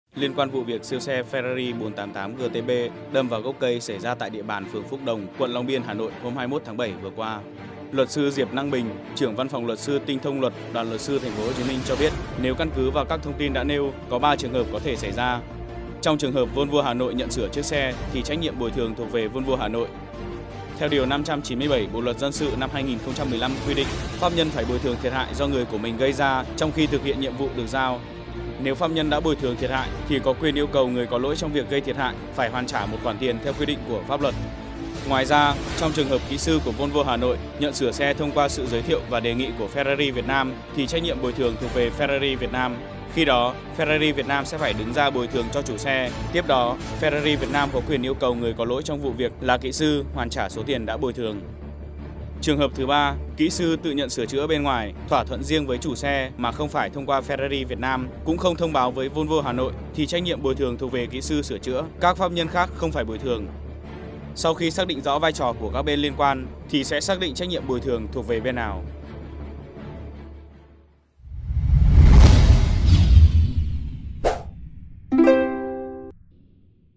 Ai phải b.ồi th.ư.ờng vụ siêu xe Ferrari 488 đ.â.m gốc cây? | VTC14
VTC14 | Liên quan vụ việc siêu xe Ferrari 488 GTB đ.â.m vào gốc cây xảy ra tại địa bàn phường Phúc Đồng, quận Long Biên, Hà Nội hôm 21/7 vừa qua. Luật sư Diệp Năng Bình (Trưởng Văn phòng luật sư Tinh Thông Luật, Đoàn luật sư TP HCM cho biết, nếu căn cứ vào các thông tin đã nêu, có 3 trường hợp có thể xảy ra.